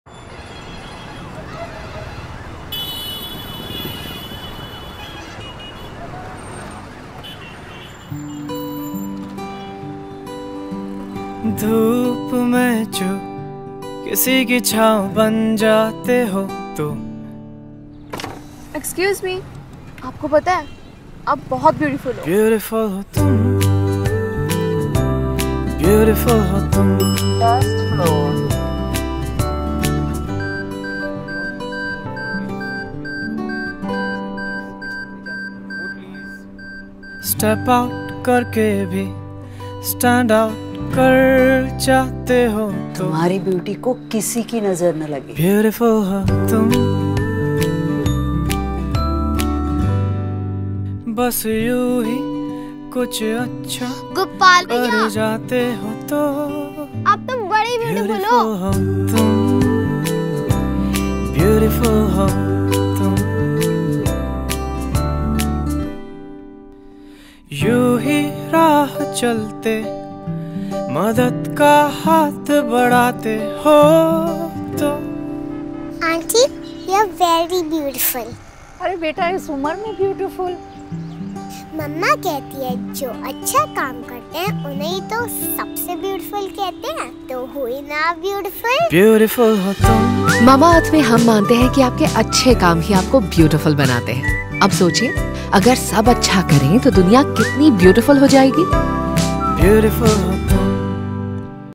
Find out what makes you beautiful | Mamaearth
At mamaearth, we believe that your good acts are what make you beautiful. A simple gesture, a kind word, or an everyday act, all bring out your true beauty.

So the next time you see someone doing a good act, let them know that they're beautiful.